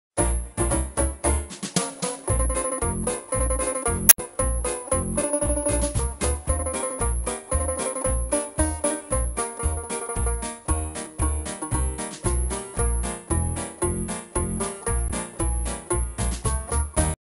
Flamingo Joke
Final project for the "Create an Animated Idiom" tutorial to learn how to use animation and chroma key features in Frames.